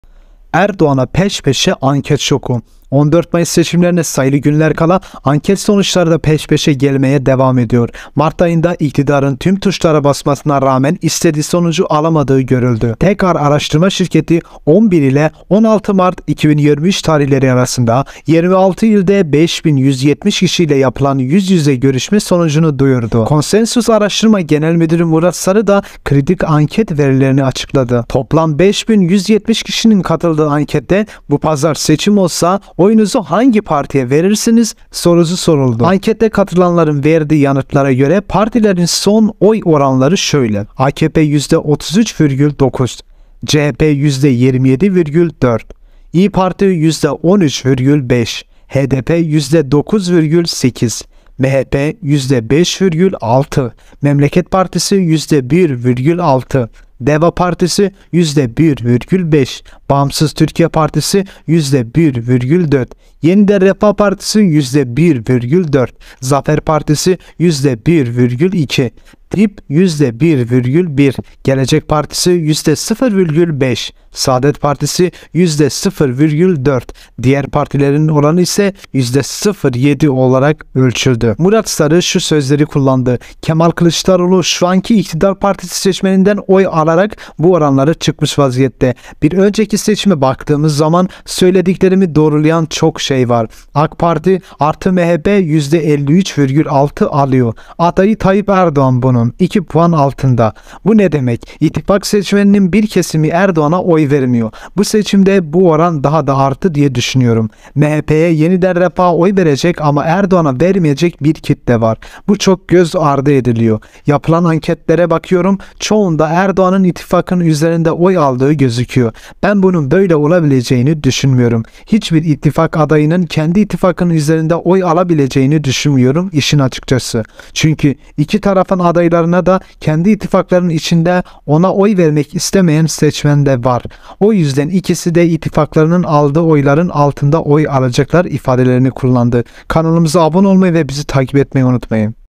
sondakika Erdoğan'a peş peşe şok! AÇIKLADILAR..
sondakika podcast Konsensus Araştırma Şirketi sahibi ve Genel Müdürü Murat Sarı, cumhurbaşkanlığı ve milletvekili seçimlerine iki aydan az bir süre kalmışken son durumla ilgili gözlemlerini Sözcü yazarı Ruhat Mengi'ye anlattı. Sarı, Cumhur İttifakı seçmeninin bir kısmının Cumhurbaşkanı Recep Tayyip Erdoğan'a oy vermediğini ve önümüzdeki seçimde bu oranın daha da artacağını düşündüğünü söyledi.

Hepinize Teşekkürederiz!